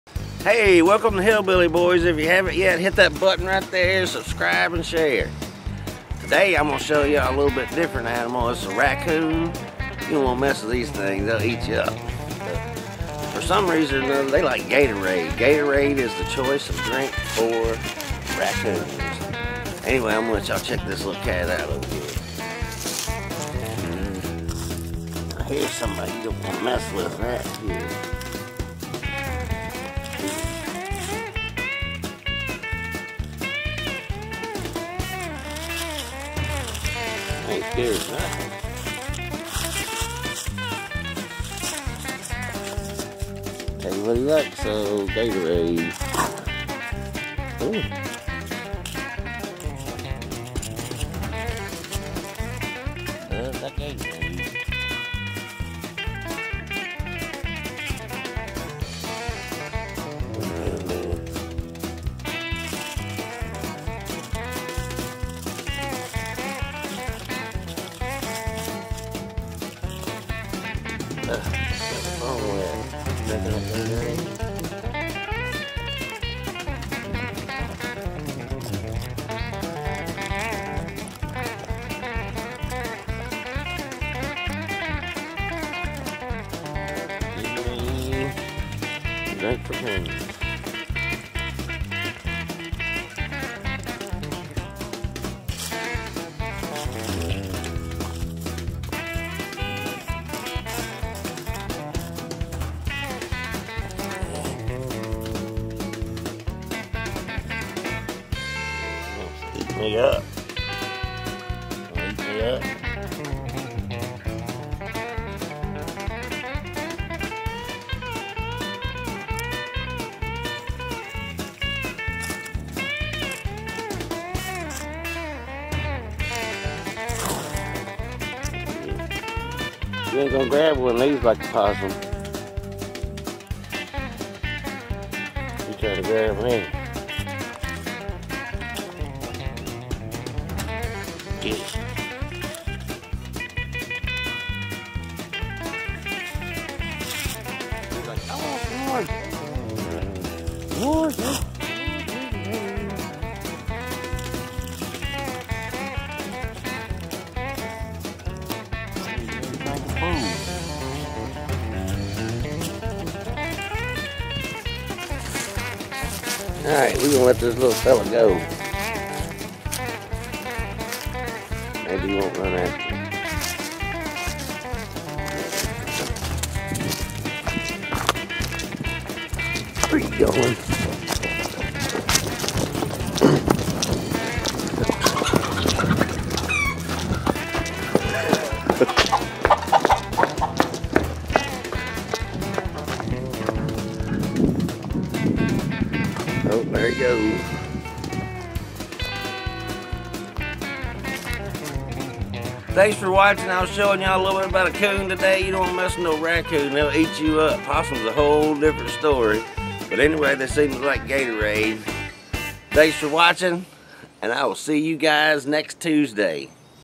Raccoons love Gatorade!
I caught this little guy in my trap and gave him some water and he wouldn't drink it. I gave him Gatorade and he went nuts! Watch him drink up some Lemon Lime Gatorade! It's the choice of beverage for Raccoons.

What's your experience with Raccoons? Let me know about it in the comments below?